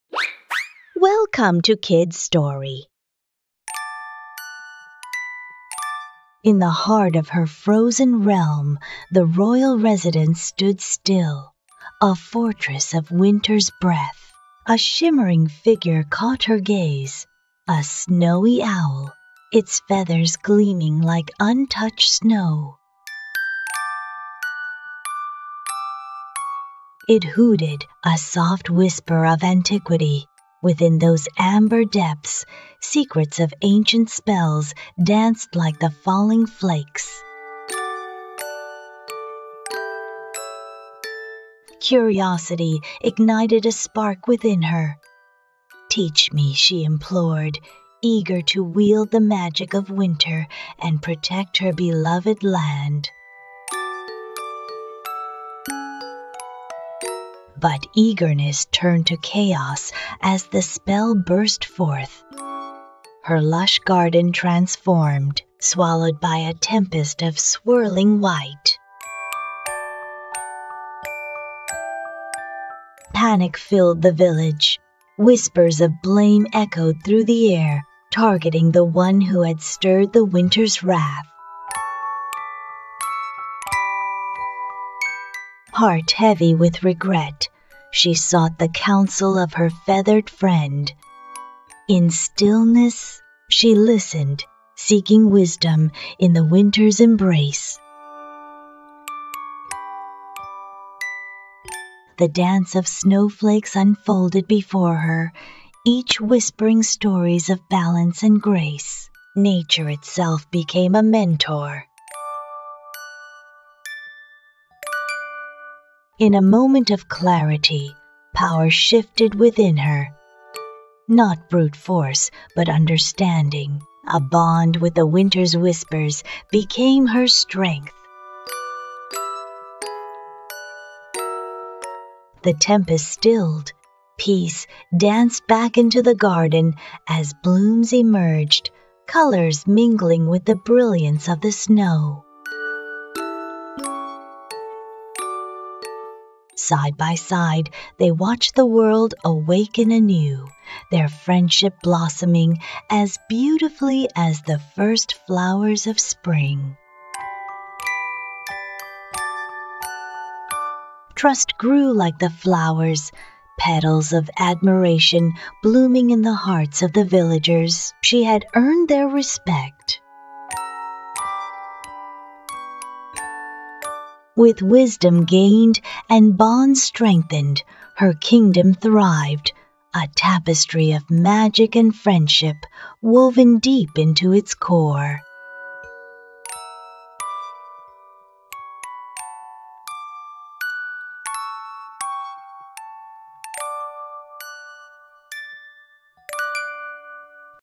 Princess Elsa and the Winter Owl: A Tale of Wisdom and Friendship | Fairy Tales | Story for Bedtime
Join Princess Elsa on a magical adventure as she discovers the power of friendship, understanding, and the importance of respecting nature. When she encounters a wise old owl who possesses ancient secrets, she must use all her courage and determination to learn from him and protect her kingdom. This heartwarming tale is filled with inspiration, hope, and the transformative power of the human spirit.